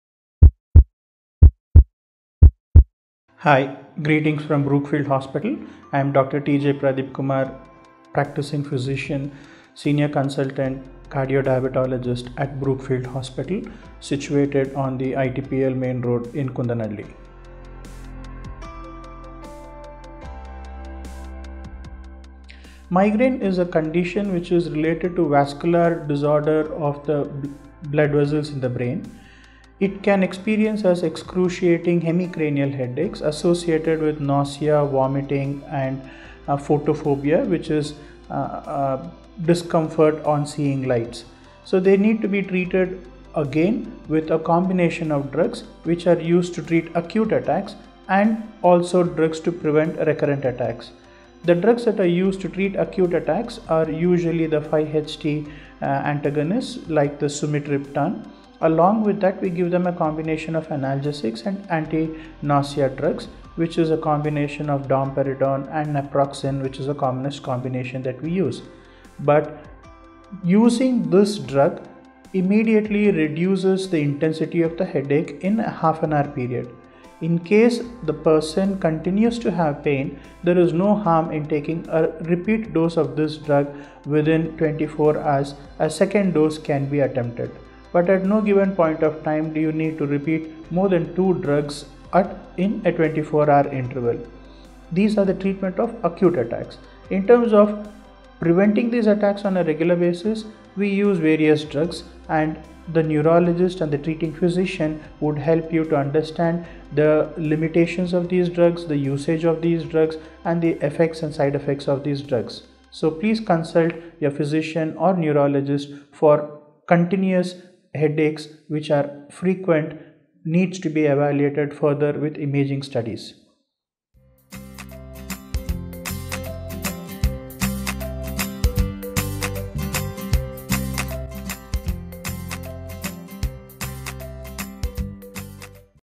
Is taking 2 doses of combination drug at one time for Migraine safe? - Dr. Pradeep Kumar T J
Migraine is a condition which is related to vascular disorder of the blood vessels in the brain. It can experience excruciating hemicranial headaches associated with nausea, vomiting and photophobia, which is a discomfort on seeing lights. So they need to be treated again with a combination of drugs which are used to heal acute attacks and also drugs to prevent recurrent attacks. The drugs that are used to treat acute attacks are usually the 5 HT antagonists like sumatriptan, along with that we give a combination of analgesics, anti-nausea drugs, which is a combination domperidone and naproxen, which is the commonest combination that we use. But using this drug immediately reduces the intensity of the headache in half an hour period. In case the patients continue to have pain, there is no harm in taking a repeat dose of drug within 24 hours, a second dosage can be attempted. But at no given point of time, you need to repeat more than 2 drugs in a 24 hour interval. These are the treatment of acute attacks. In terms of preventing the attacks on a regular basis, we use a various drugs and the neurologist and the treating physician would help you to understand the limitation of these drugs, the usage of these drugs and the effects and the side effects of these drugs. So please consult your physician or neurologist for continuous headaches which are frequent, needs to be evaluated further with imaging studies.